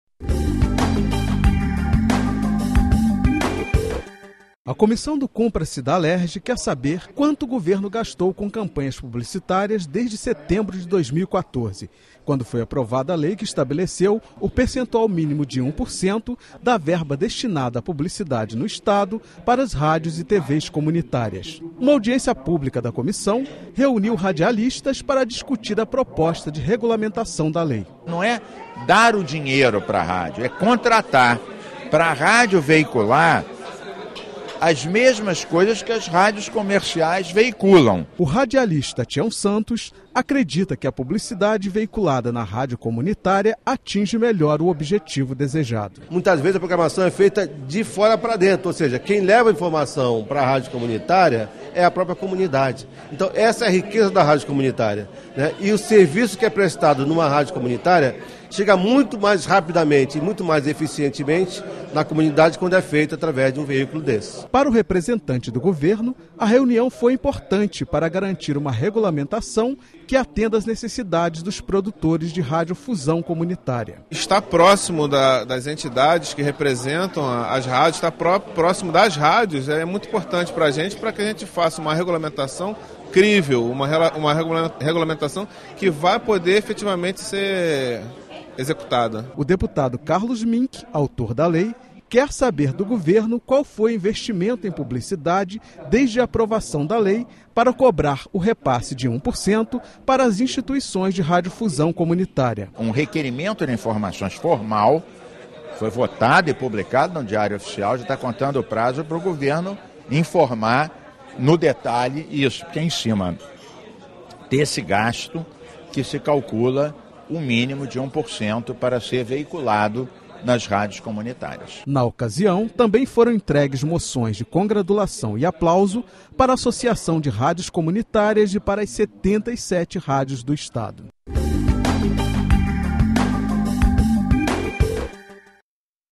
Rádios e TVs comunitárias
A comissão do Cumpra-se da Alerj quer saber quanto o Governo gastou com campanhas publicitárias desde setembro de 2014, quando foi aprovada a lei que estabeleceu o percentual mínimo de 1% da verba destinada para a publicidade no estado para as rádios e tvs comunitárias.